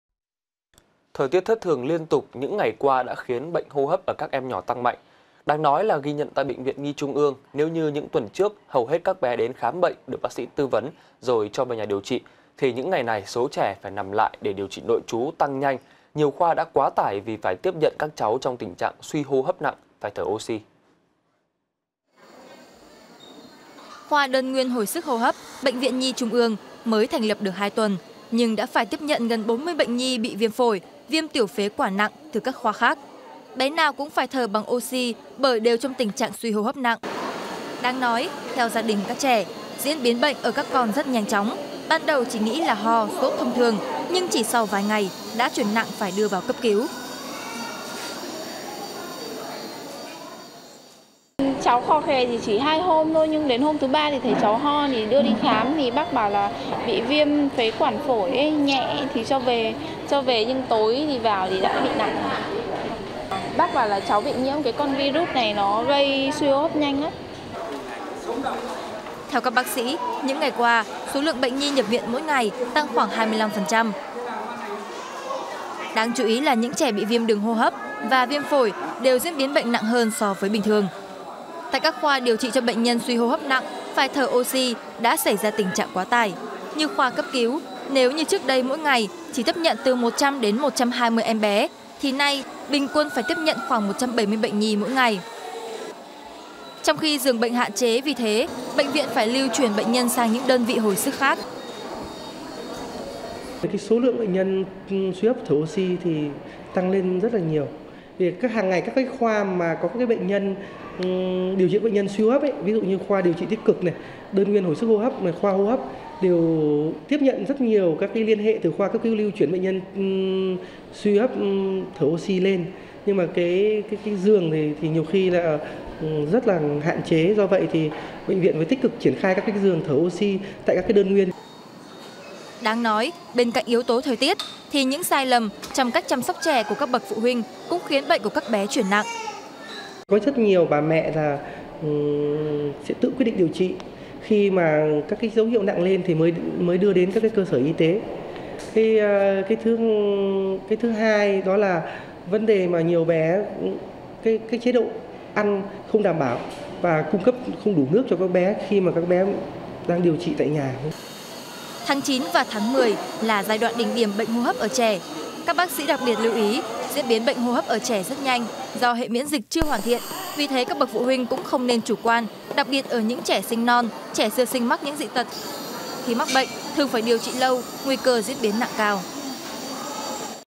(VTC14)_Giao mùa, bệnh nhi suy hô hấp phải thở oxi tăng mạnh
(VTC14) - Thời tiết thất thường liên tục những ngày qua đã khiến bệnh hô hấp ở các em nhỏ tăng mạnh. Đáng nói, ghi nhận tại Bệnh viện Nhi Trung ương, nếu như những tuần trước, hầu hết các bé đến khám bệnh được bác sĩ tư vấn rồi về nhà điều trị, thì những ngày này, số trẻ phải nằm lại để điều trị nội trú tăng nhanh, nhiều khoa đã quá tải vì phải tiếp nhận các cháu trong tình trạng suy hô hấp nặng phải thở oxi.